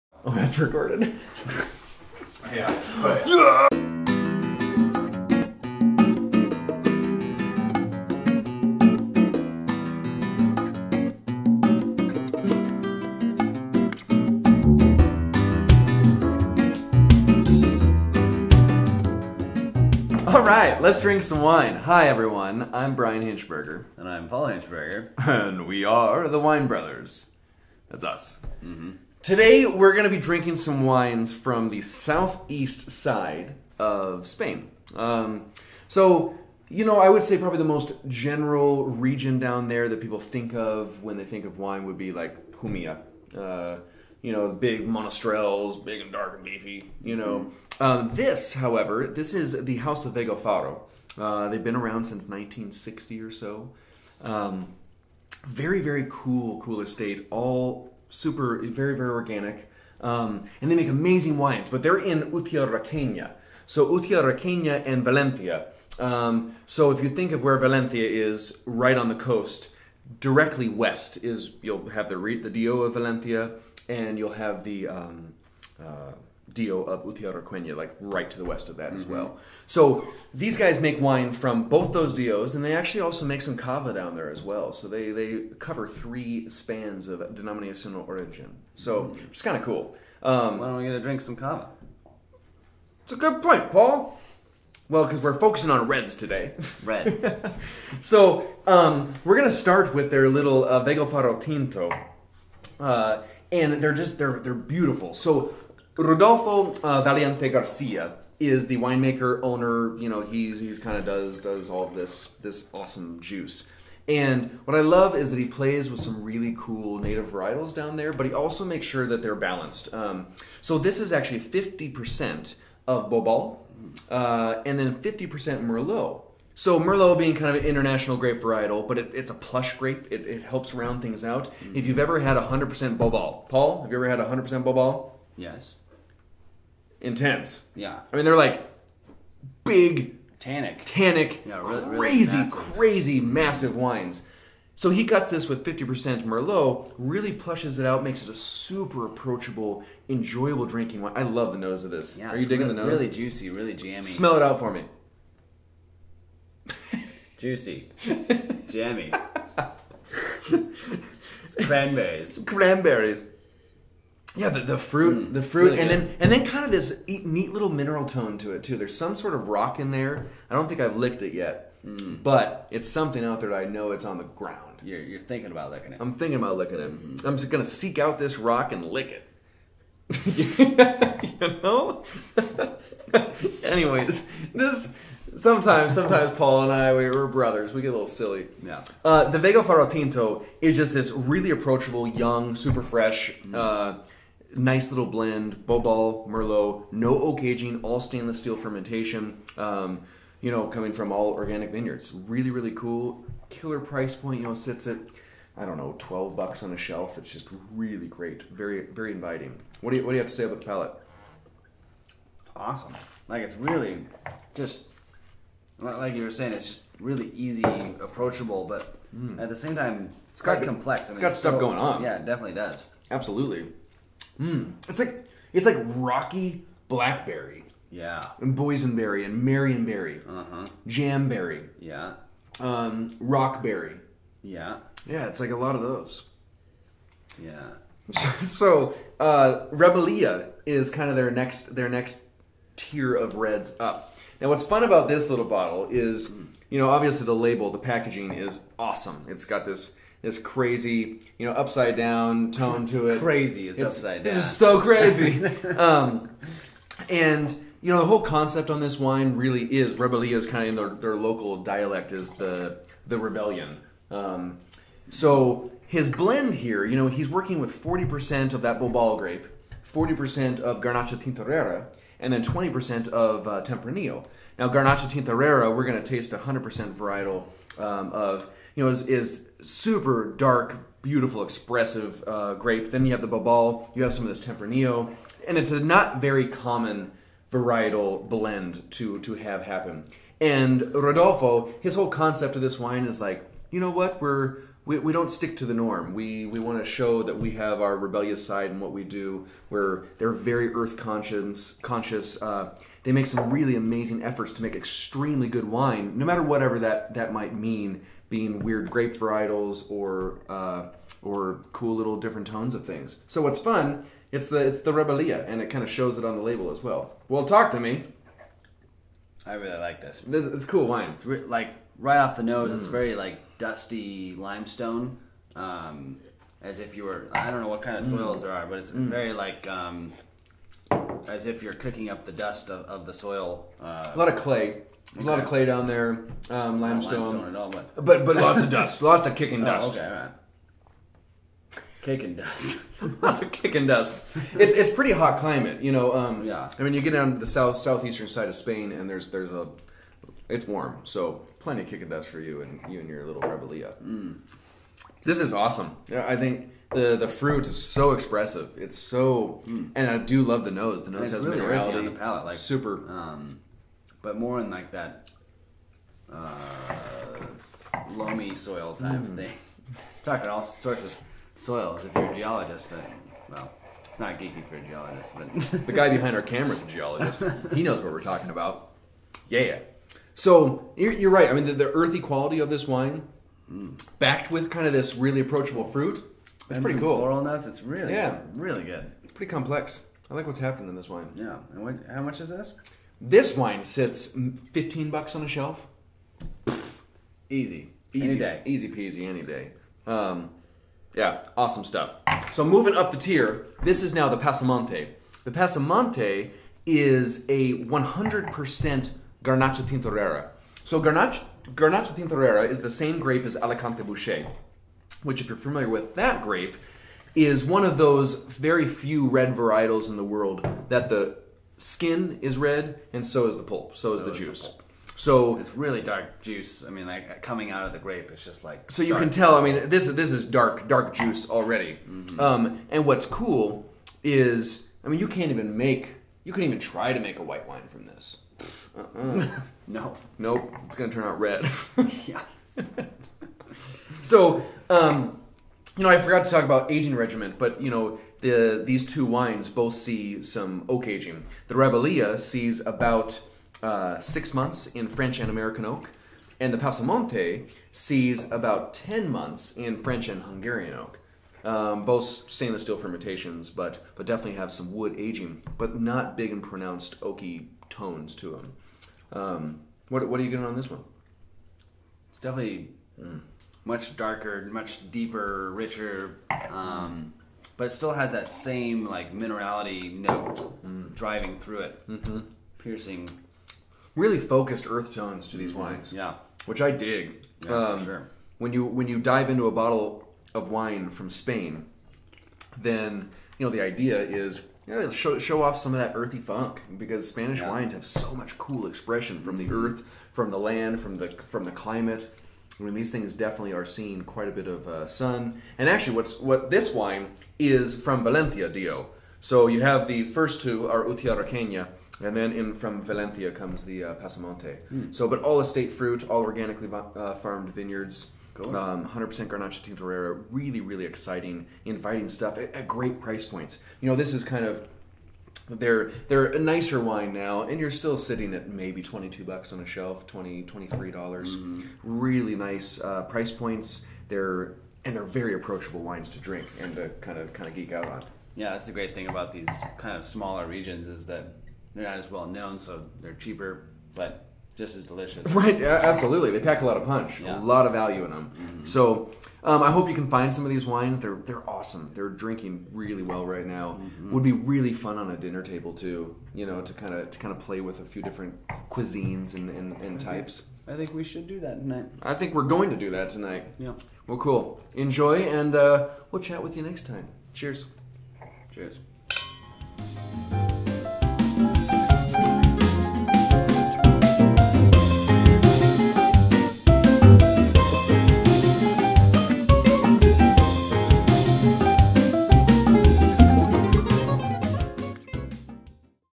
The Wine Brothers - Wines from Valencia
The Wine Brothers present Bodegas Vegalfaro, an amazing organic estate from the Valencia region of Southeast Spain.  A cool use of Bobal, Garnacha Tintorera, Tempranillo... lovely yet intense wines!